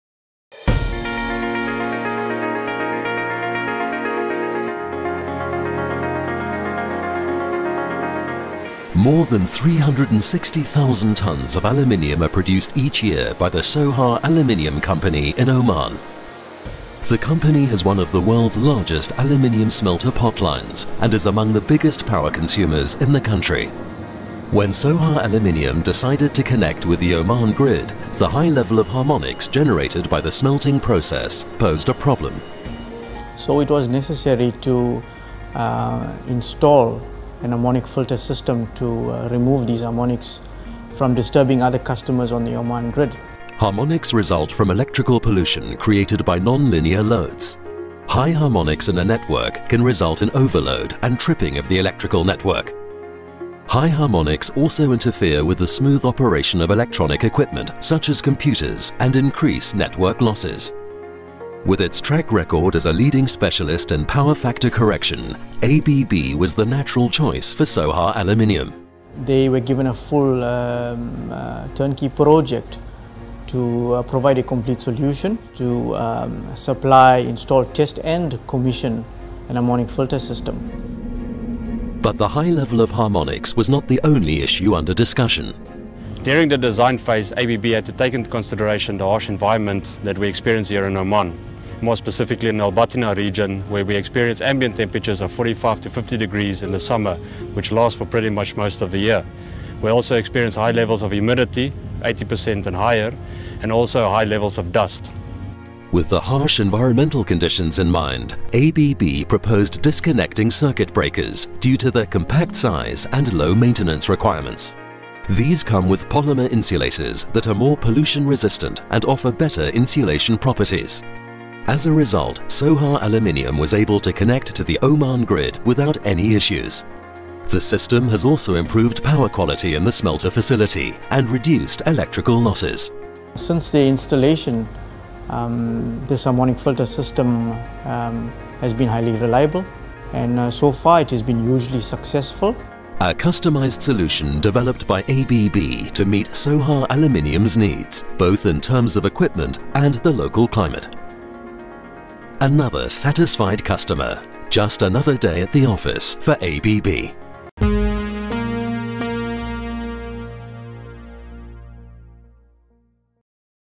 ABB Enhancing power quality Sohar
Expertise in power quality harmonic filter solutions is one of ABB's core competencies. Sohar Aluminium in Oman is one of the world’s largest aluminium smelter facilities. It employs a customized power quality solution from ABB to ensure grid compliance and enhance energy efficiency.